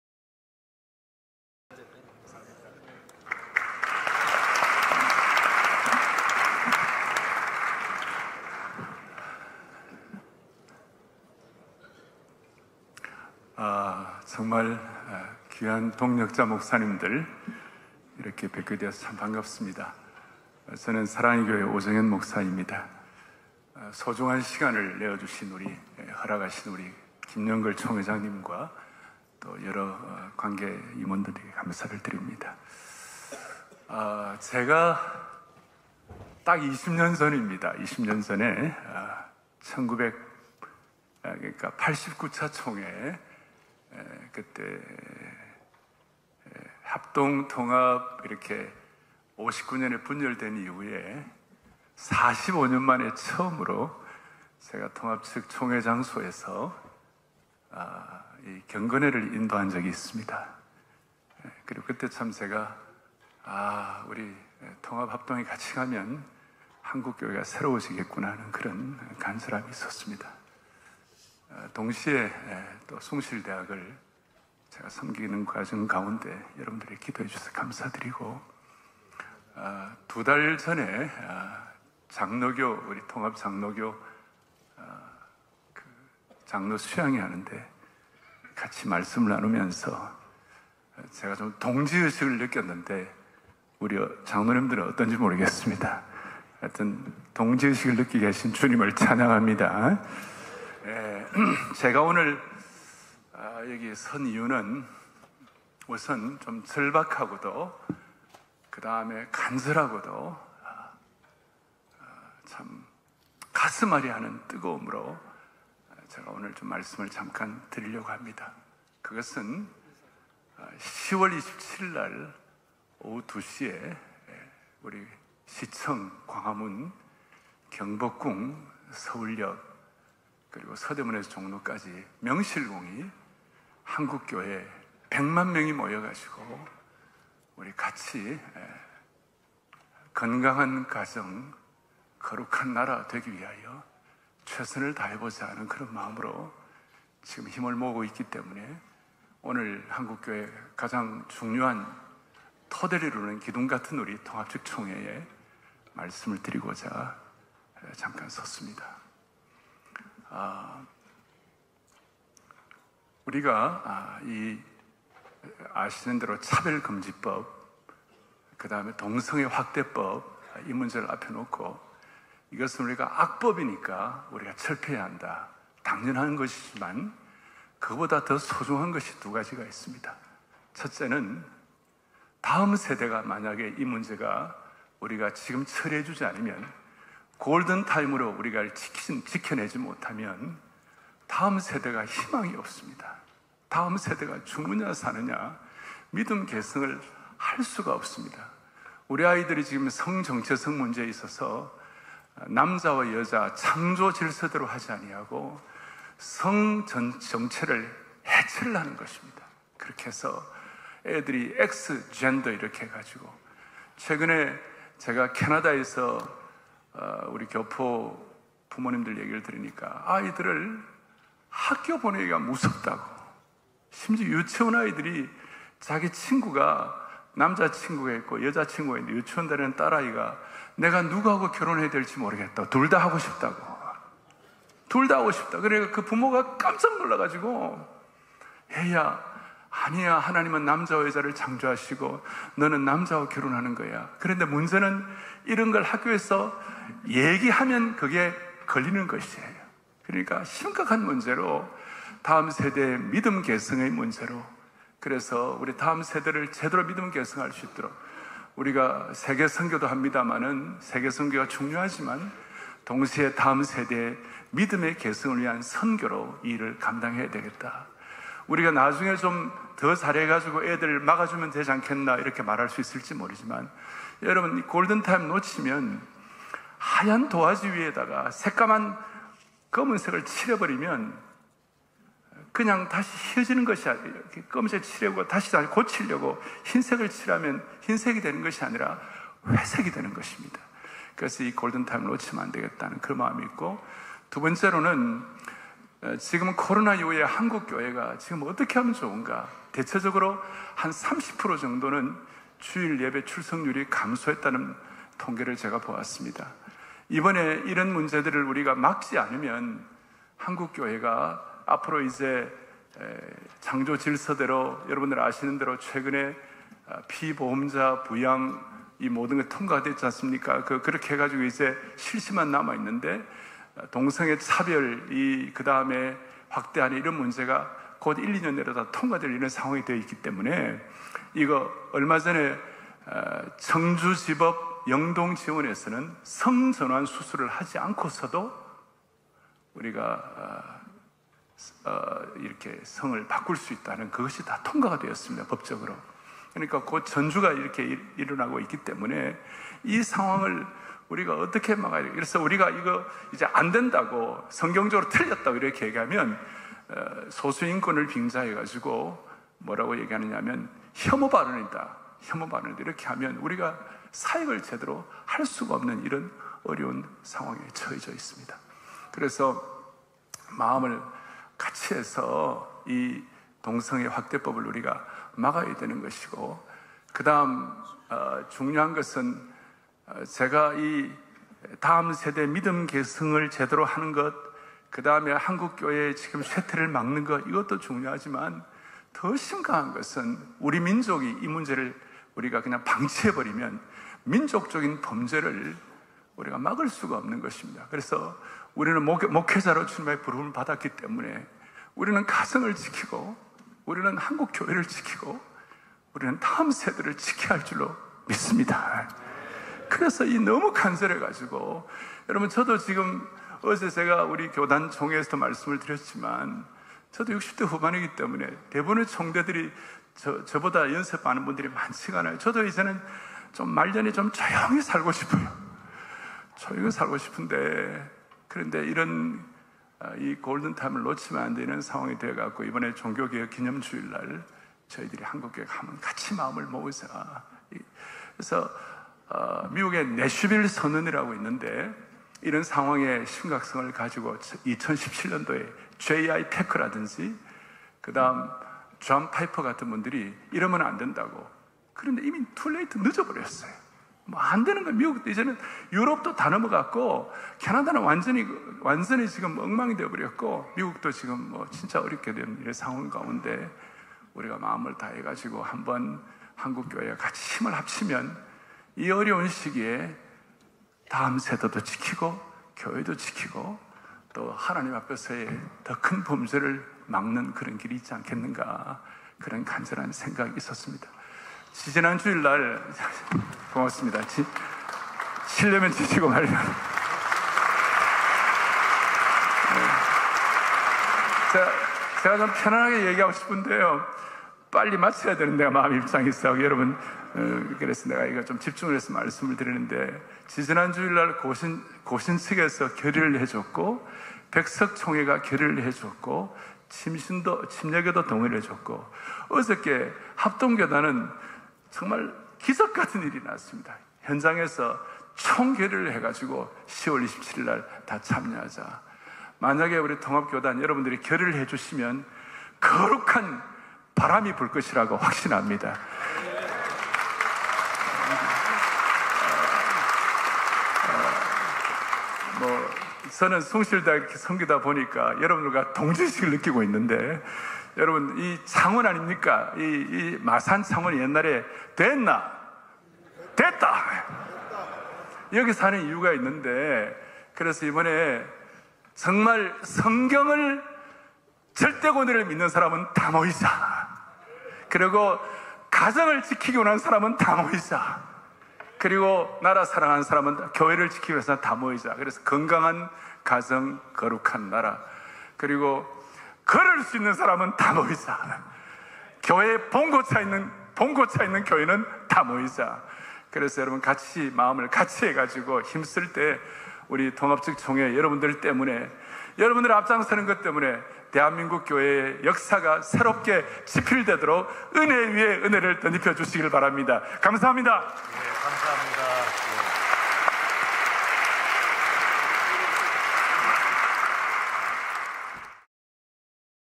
통합 총회에서 1027 연합예배 참여를 독려하는 오정현 목사
예장 통합교단의 총회에서 1027 연합예배를 위해 간절히 호소하는
사랑의 교회 오정현목사의 영상입니다.
1027 연합예배를 위해 함께 기도합시다.
1027에 다 모이자!!

1027 연합예배 & 큰 기도회
시간: 2024년 10월 27일 주일 오후 2시~5시
장소: 광화문-서울시청 일대